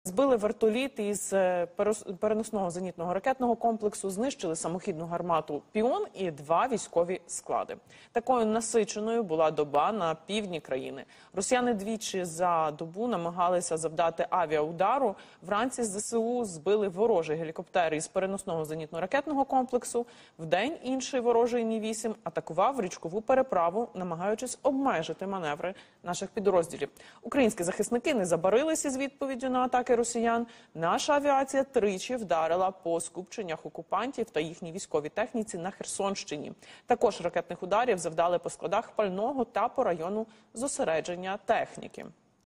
Українські захисники збили вертоліт із ПЗРК, знищили "Піон" і 2 військові склади
За добу українські підрозділи завдали значних втрат загарбникам.